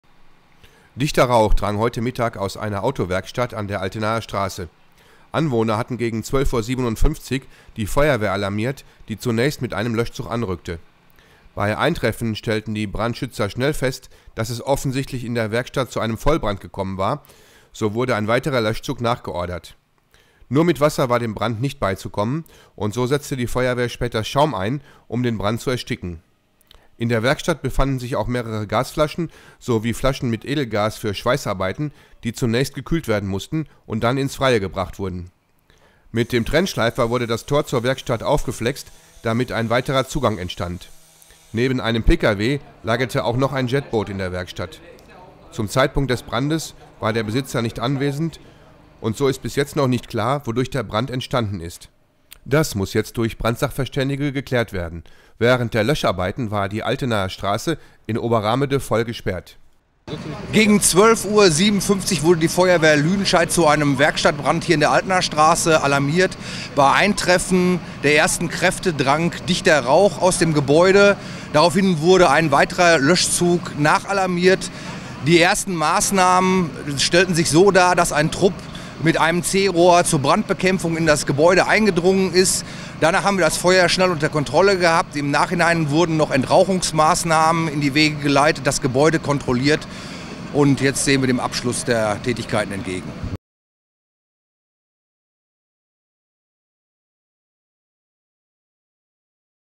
Werkstatt brennt an der Altenaer Straße
Dichter Rauch drang heute Mittag aus einer Autowerkstatt an der Altenaer Straße. Anwohner hatten gegen 12.57 die Feuerwehr alarmiert, die zunächst mit einem Löschzug anrückte.  Bei Eintreffen stellten die Brandschützer schnell fest, dass es offensichtlich in der Werkstatt brennt und so wurde ein weiter Löschzug nachgeordert. Nur mit Wasser war dem Brand nicht beizukommen und so setzte die Feuerwehr später Schaum ein, um den Brand zu ersticken. In der Werkstatt befanden sich auch mehrere Gasflaschen sowie Flachen mit Edelgasen für Schweissarbeiten, die zunächst gekühlt wurden und dann ins Freie gebracht wurden. Mit der Trennschleifer wurde das Tor der Werkstatt auf geflext damit ein weiterer Zugang entstand. Neben einem PKW lagerte auch noch ein Jetboot in der Werkstatt. Zum Zeitpunkt des Brandes war der Besitzer nicht anwesend und so ist bis jetzt noch nicht klar, wodurch der Brand entstanden ist. Das muss jetzt durch Brandsachverständige geklärt werden. Während der Löscharbeiten war die Altenaer Straße in Oberrahmede voll gesperrt.